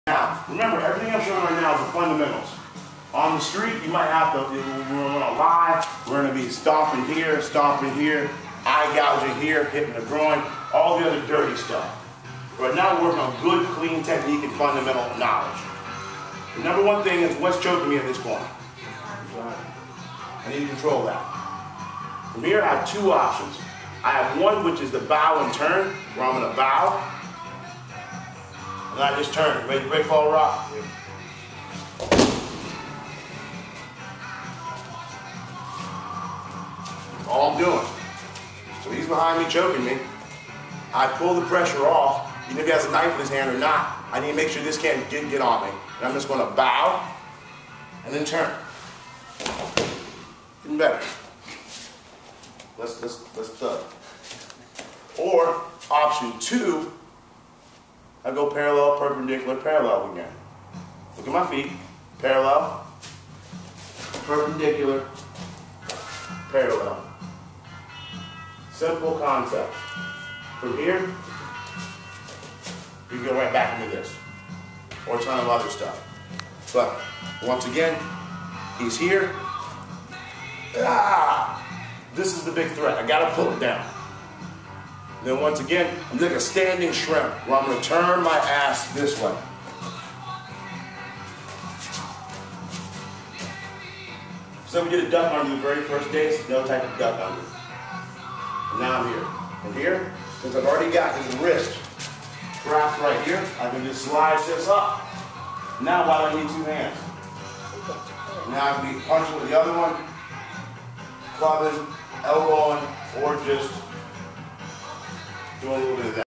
Self Defense 101: Standing Rear Choke/Headlock Defense 1 Chantilly / South Riding
Excerpts from Day 3 of my Self Defense Camp
Standing Rear Choke/Headlock defense